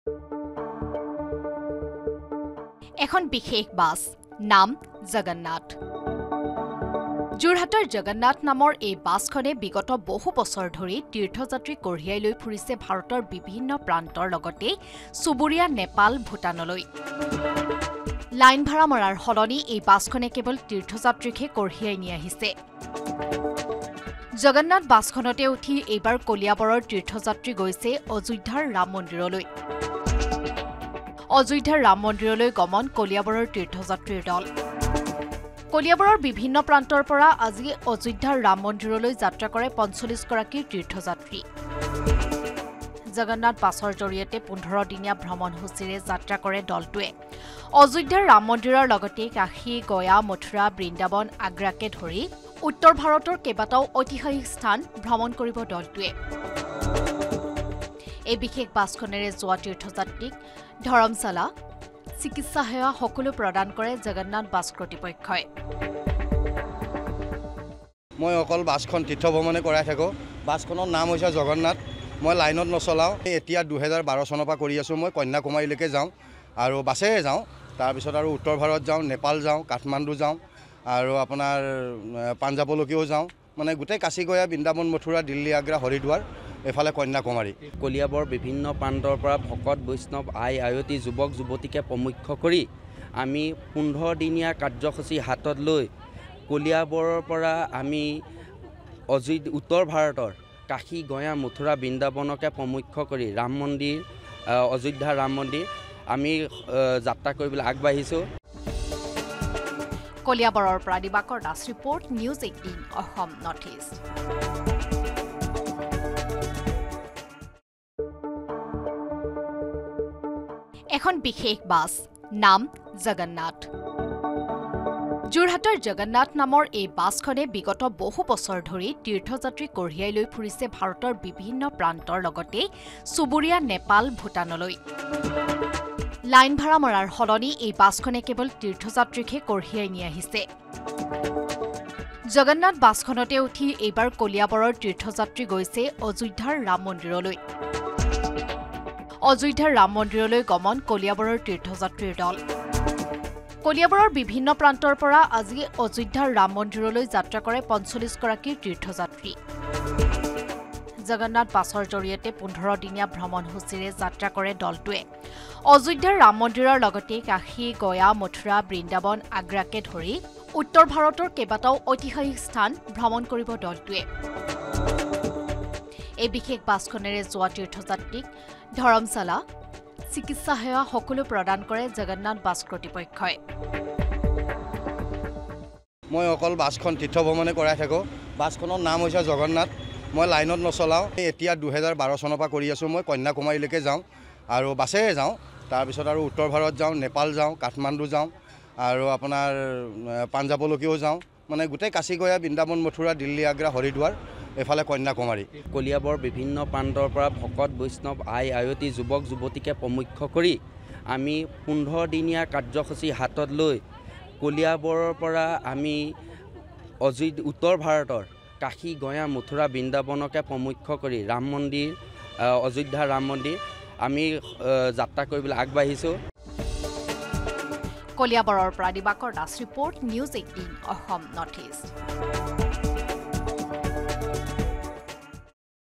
Bus For Tirth Yatri : এখন বিশেষ বাছ। যিখন বাছ কেৱল তীৰ্থযাত্ৰালৈ যায় | N18V
Bus For Tirth Yatri | এখন বিশেষ বাছ। যিখন বাছ কেৱল তীৰ্থযাত্ৰালৈ যায়। লাইন ভাড়াৰ পৰিৱৰ্তে তীৰ্থযাত্ৰাৰ ভাড়া কঢ়িওৱা এই বিশেষ বাছখন এতিয়া চৰ্চাত। এইবাৰ ‘জগন্নাথ’ নামৰ বাছখনে যাত্ৰা আৰম্ভ কৰিছে অযোধ্যাৰ ৰাম মন্দিৰলৈ।